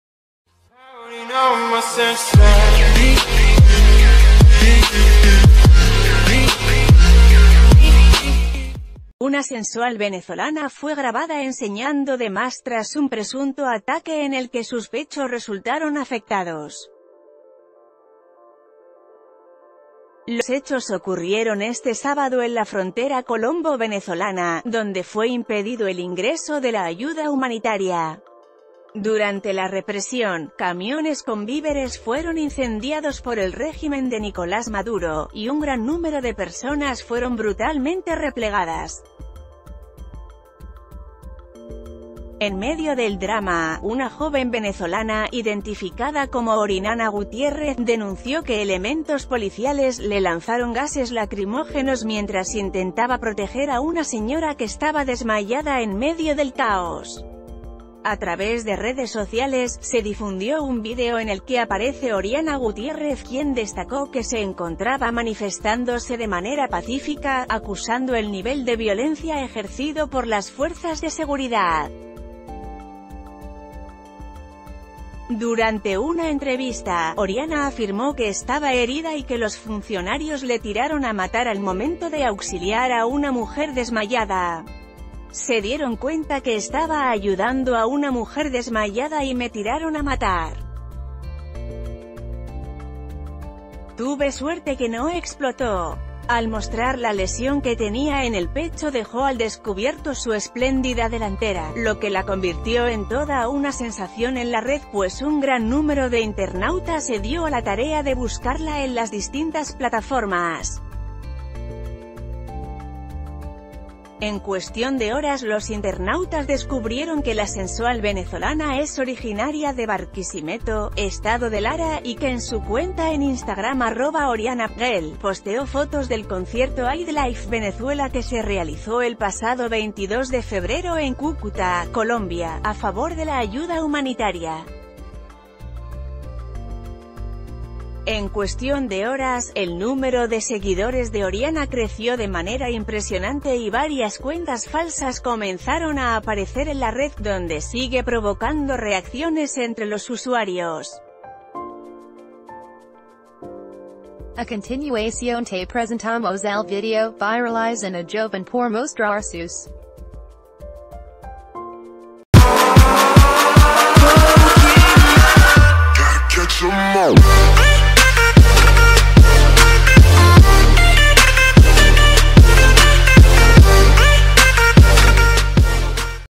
Graban a venezolana enseñando todo tras presunto ataque en sus pechos
Una sensual venezolana fue grabada enseñando de más tras un presunto ataque en el que sus pechos resultaron afectados. Los hechos ocurrieron este sábado en la frontera colombo-venezolana, donde fue impedido el ingreso de la ayuda humanitaria. Durante la represión, camiones con víveres fueron incendi...